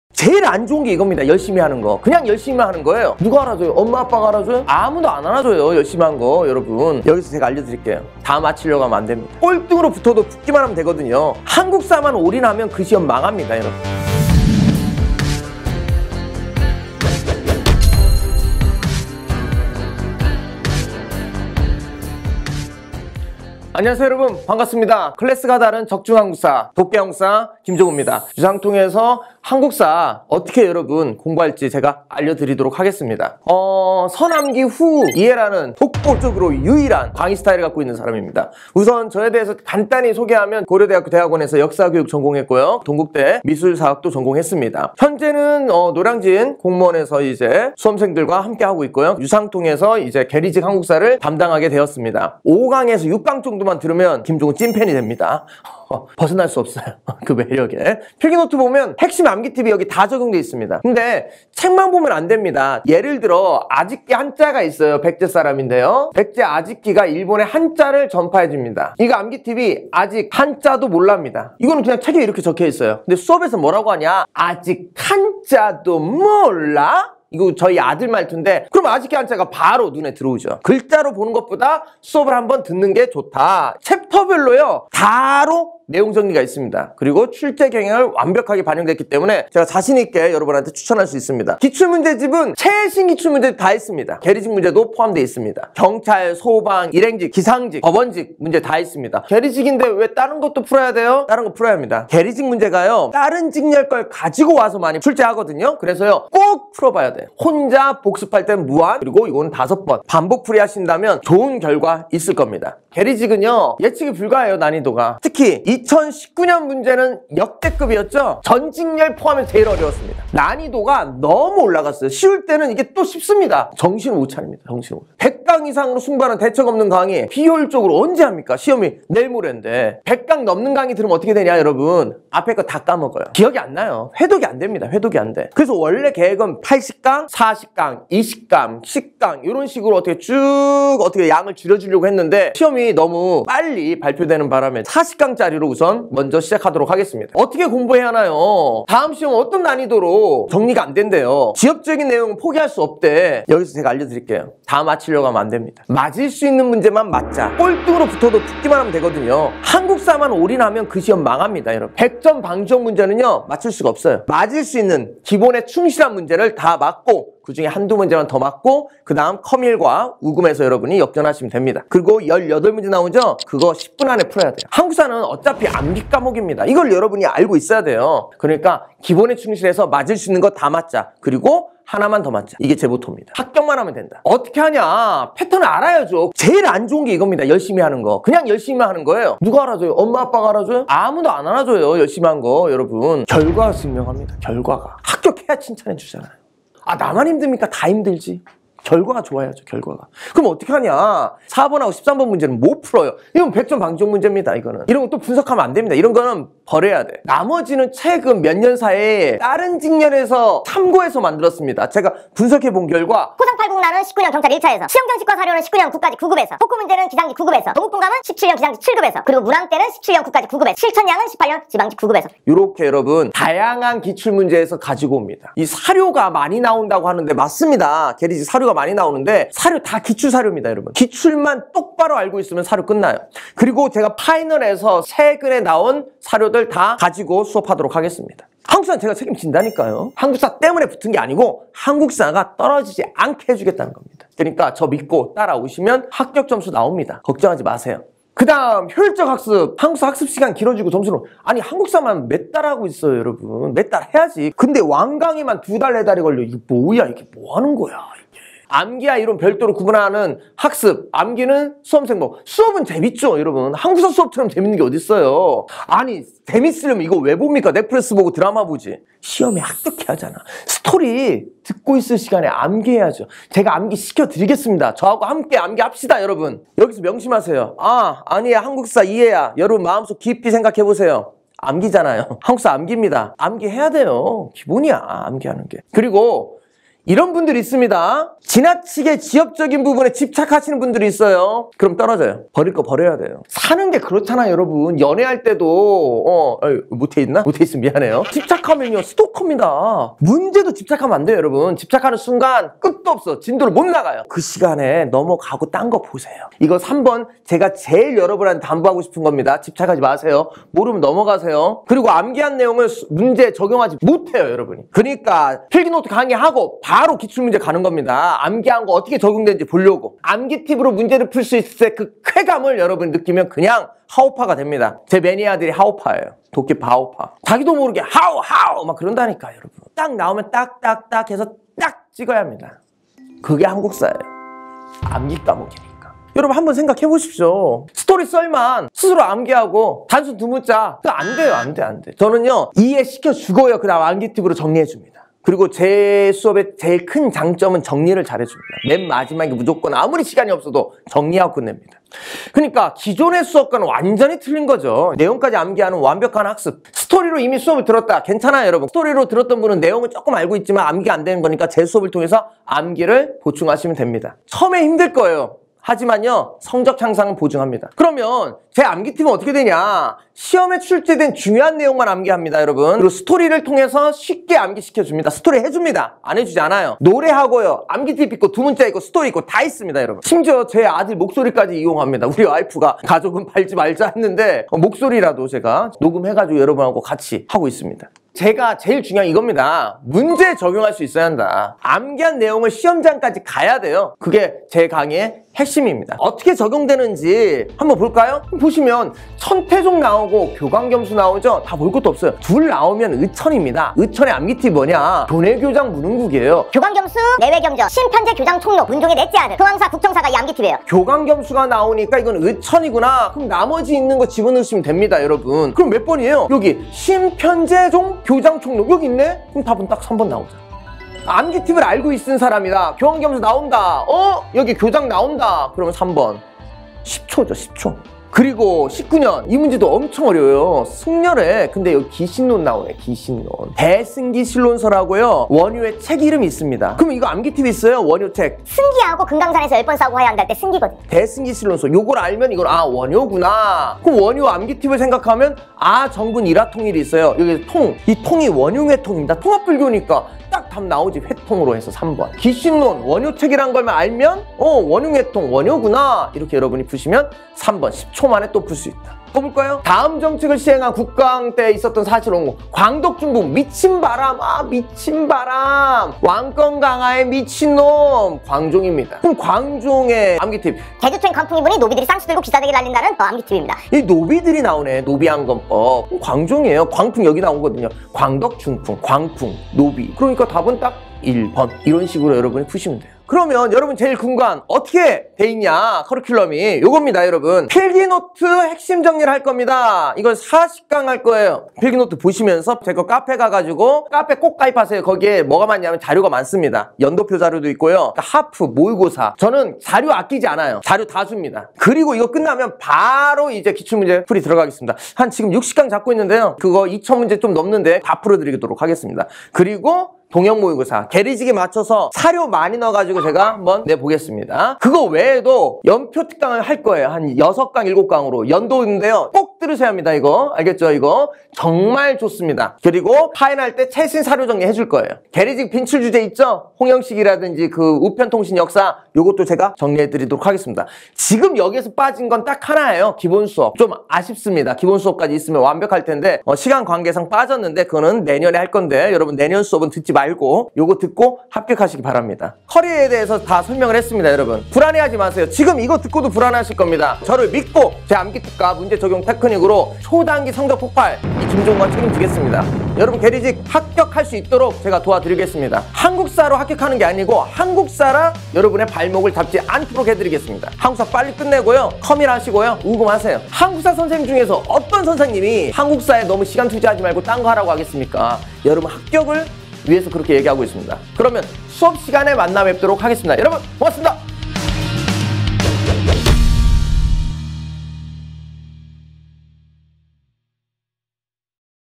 [유상통] 계리직 도끼한국사 커리큘럼 학습가이드
유상통 도끼한국사의 암기팁과 문제적용테크닉으로 계리직 한국사 초단기 성적폭발!
김종우가 책임지겠습니다.